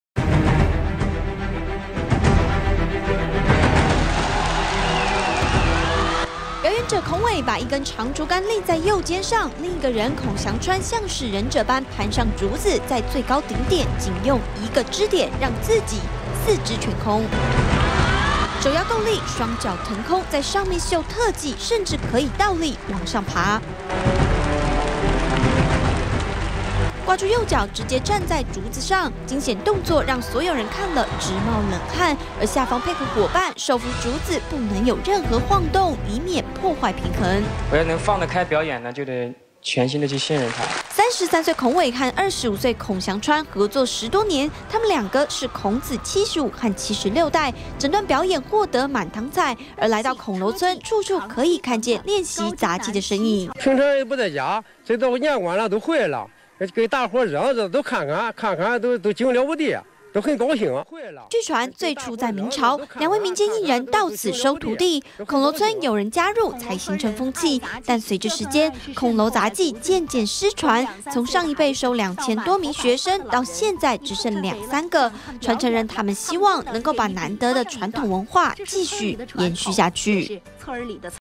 20150506中天新聞 四百年孔樓雜技 竹竿特技秀難度
被稱為孔樓絕技的民間雜技，傳承四百多年，現在絕技傳承人，走上央視舞台，特別展示絕技，表演者能徒手踩上竹子，騰空做表演，單手動作 甚至是倒立，讓所有評審看了驚呼不已。